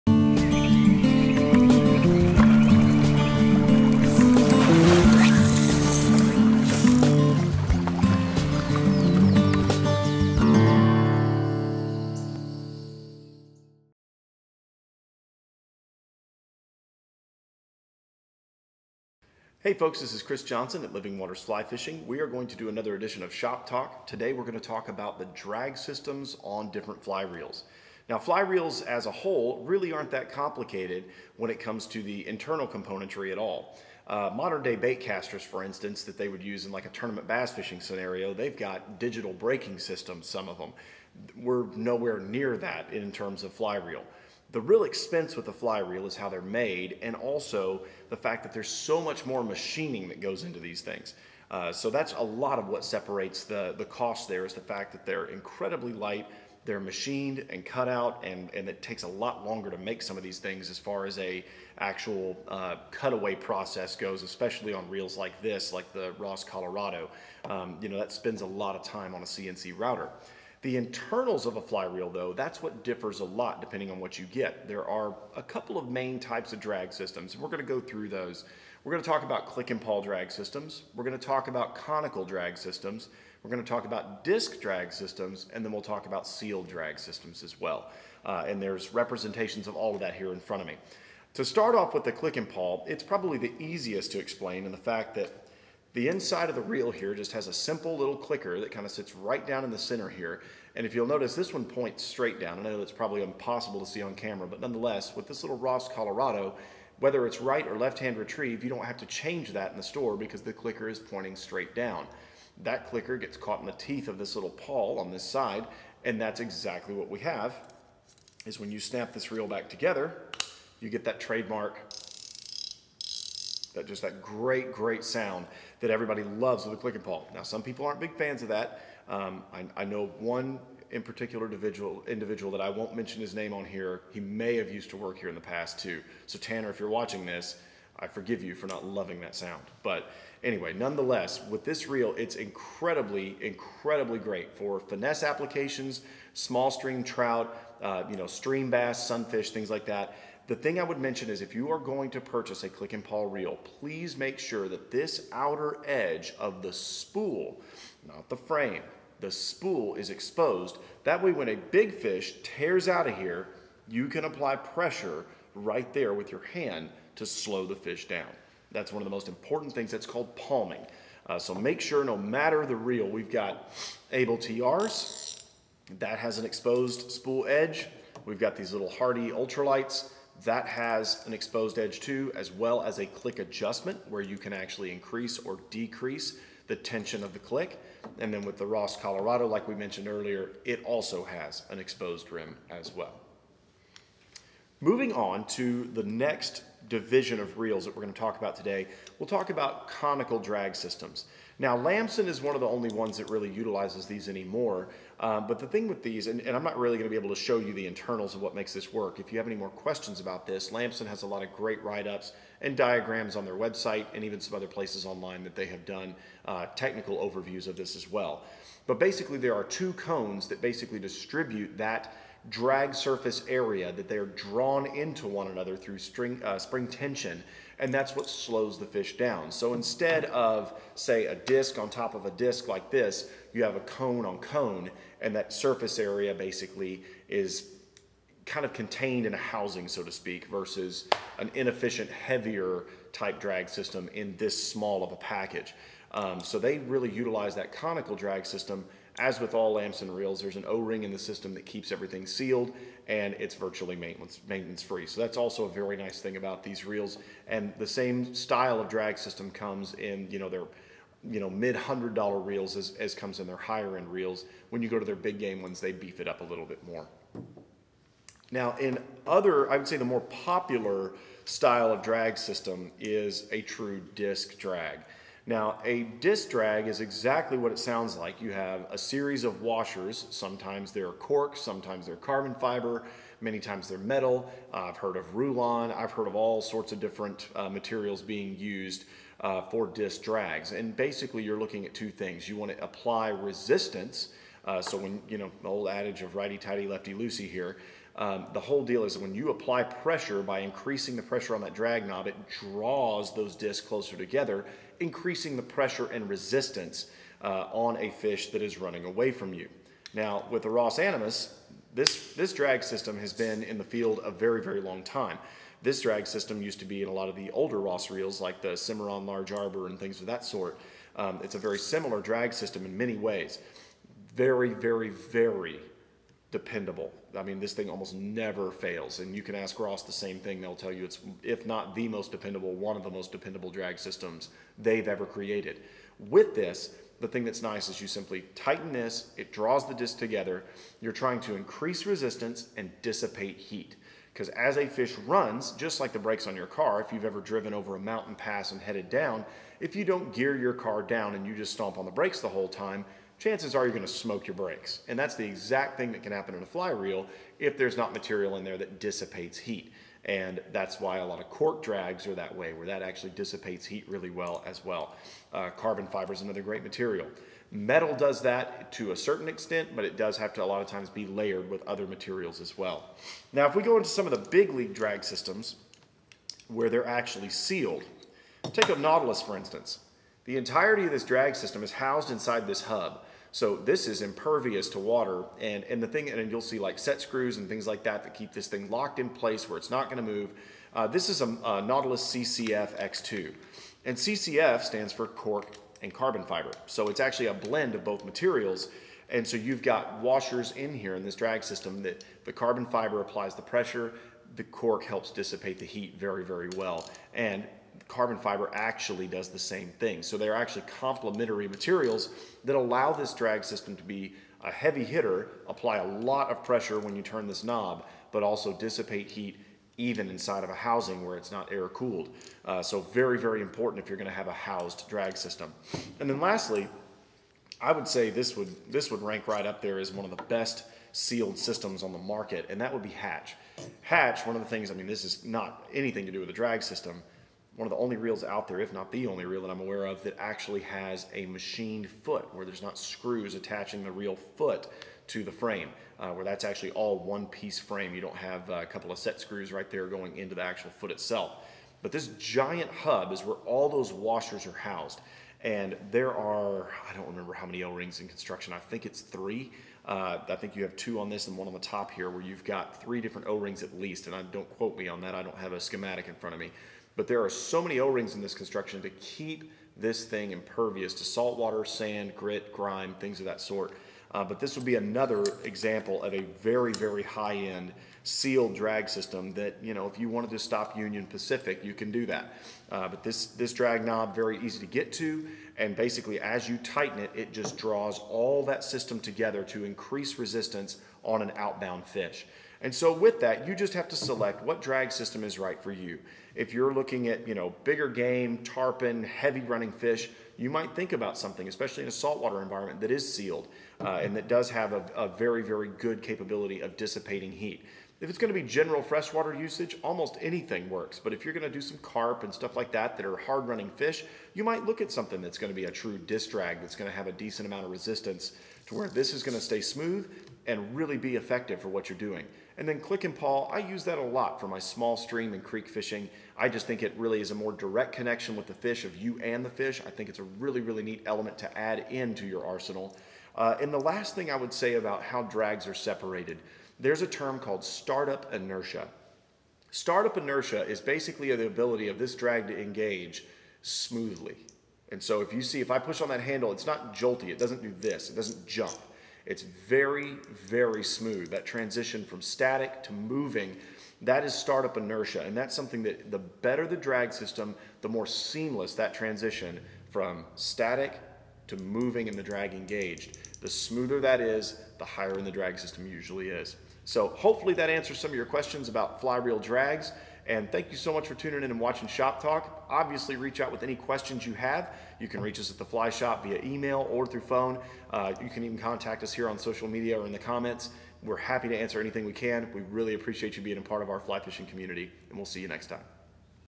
Shop Talk: Drag Systems
Fly reels are more than just line holders. They're engineering marvels which, when well-built, can last a lifetime.

In our latest Shop Talk, Living Waters' owner Chris Johnson discusses a few of the most common drag systems found in modern day fly reels, what sets each apart, and when to reach for one type of drag over another.

Abel Reels
Hatch Outdoors
Nautilus Reels
Ross Reels
Temple Fork Outfitters
Waterworks-Lamson